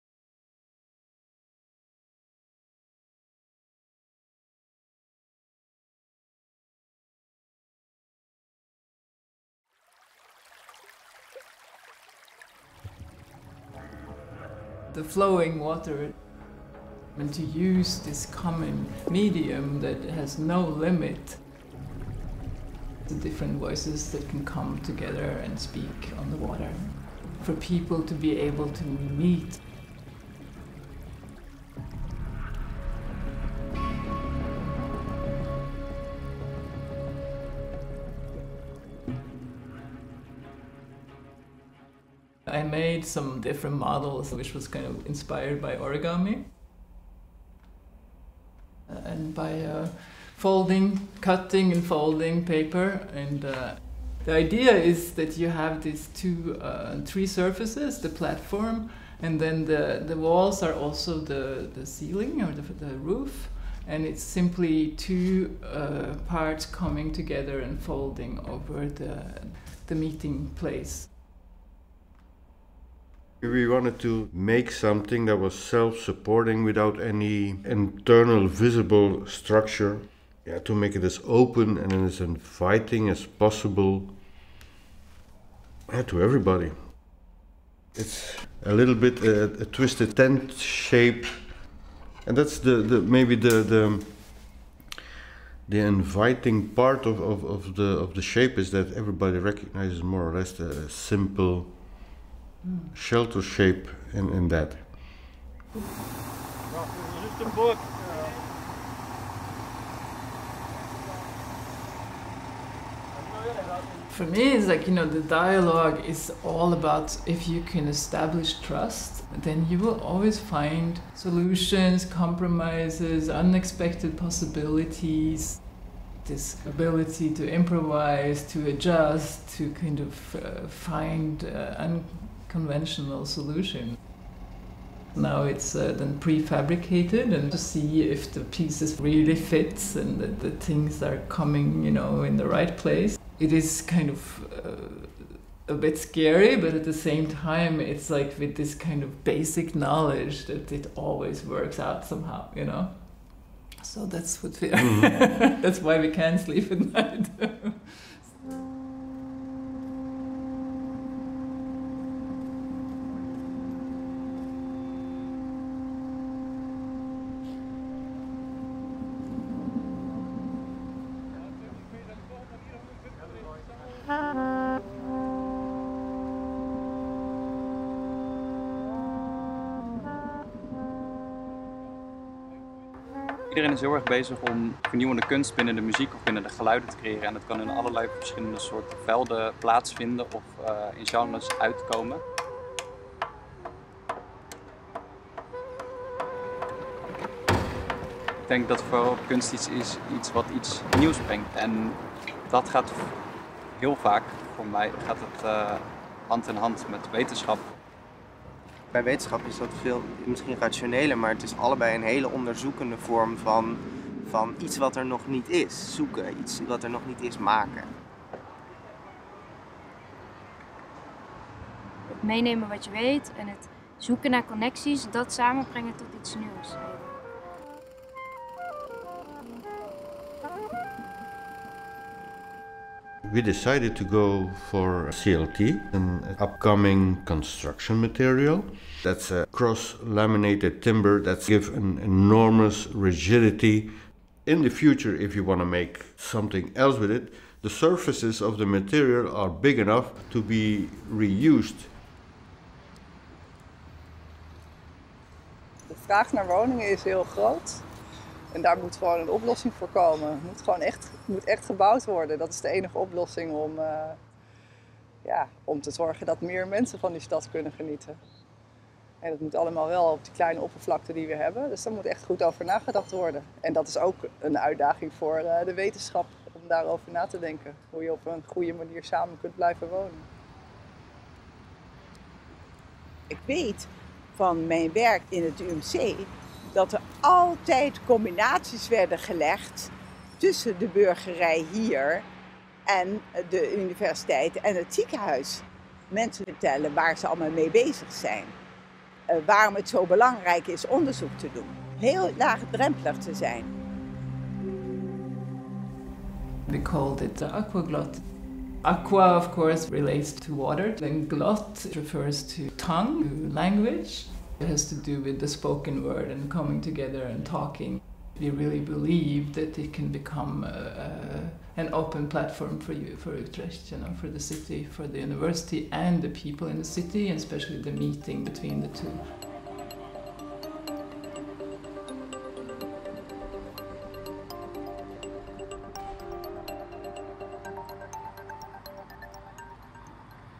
AKWAGLOT - Creating Tomorrow Together
To celebrate 385 years of science in Utrecht, a floating sustainable artwork is rising in Utrecht's Catharijnesingel. On and around this platform, Utrecht University, University Medical Centre Utrecht and various partners will organize all kinds activities from July 2021 onwards. AKWAGLOT will be the place for meeting and dialogue with residents of Utrecht.

AKWAGLOT is a creation of DE JENZ: a collaboration between artists Vibeke Jensen and Santiago De Waele.

‘AKWAGLOT – Creating Tomorrow Together’ is a production of True Heroes Films
Eugene de Groot – Production
Luce van de Weg – Editing